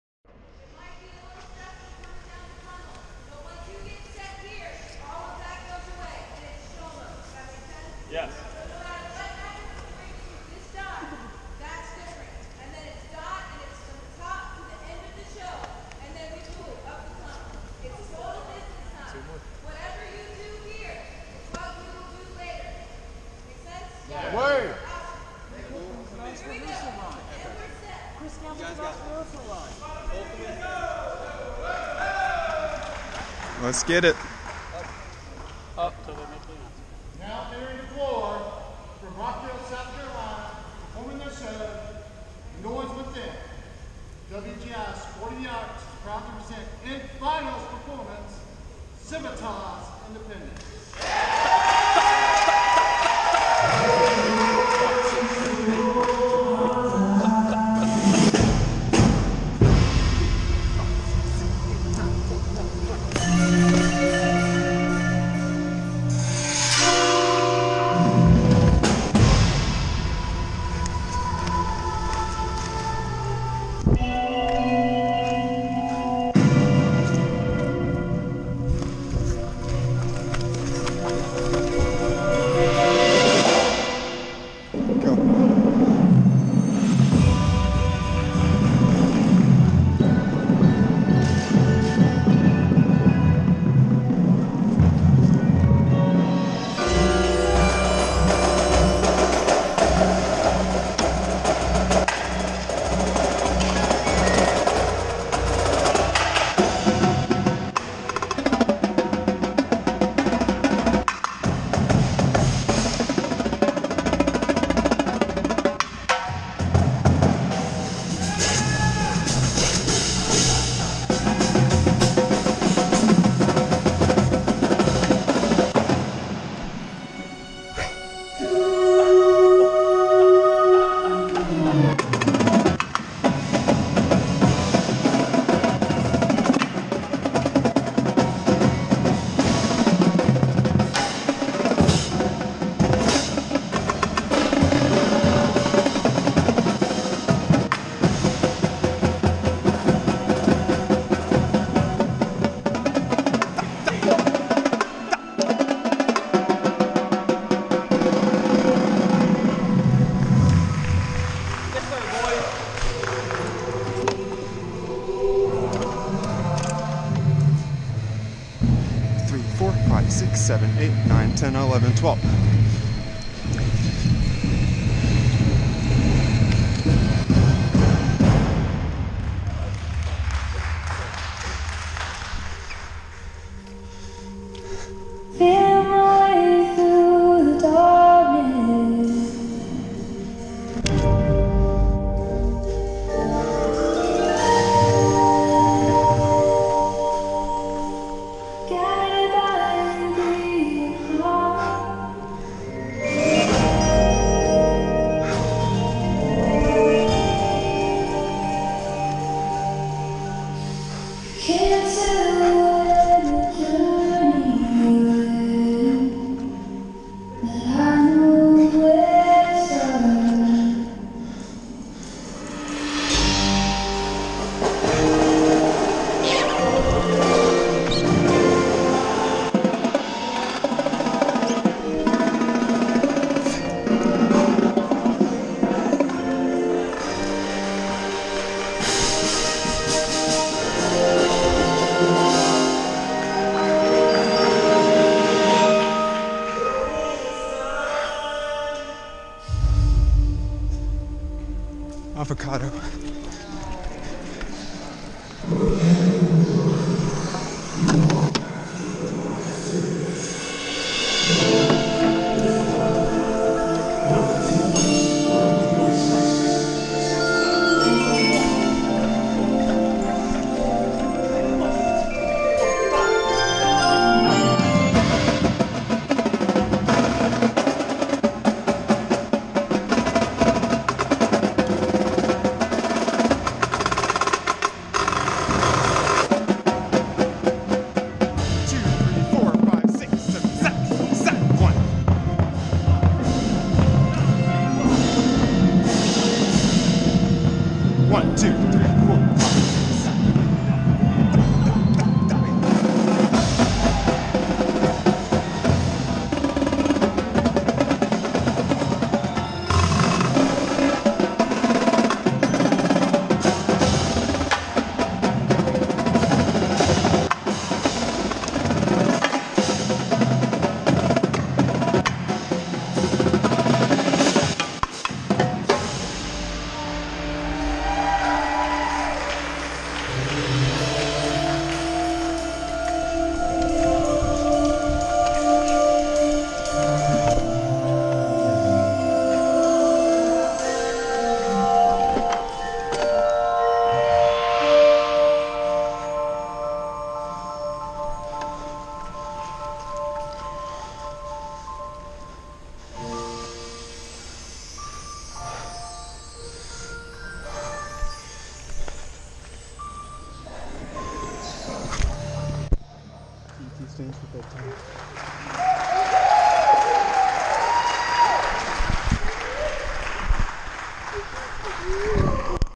Civitas Independent 2022 Quad Cam
Quad cam of Douglas Stevens at final practice before Dayton Finals.